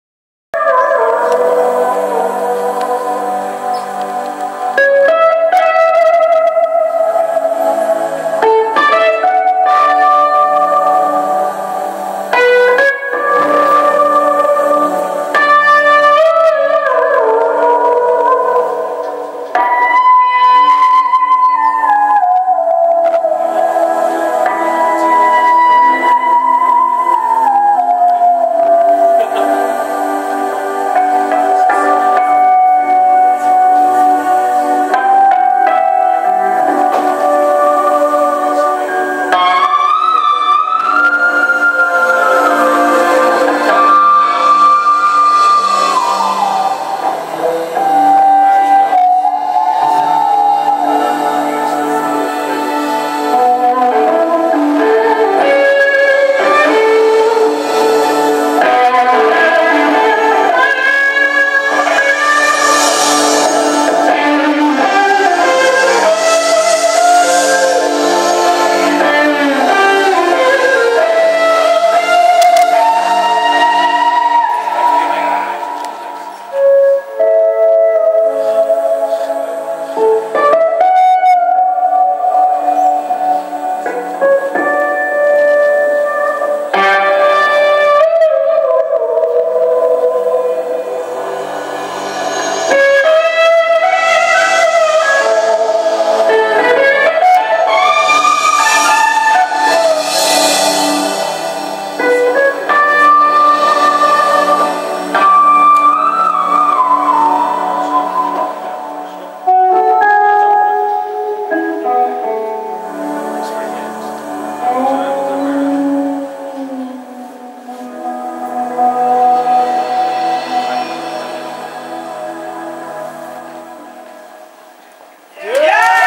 Jeff Beck - Live @ The Olympia Theatre-Dublin, Ireland.
Jeff Beck performs live, May 21st 2014.
Live at The Olympia Theatre Dublin, during the Jeff Beck 2014 World Tour.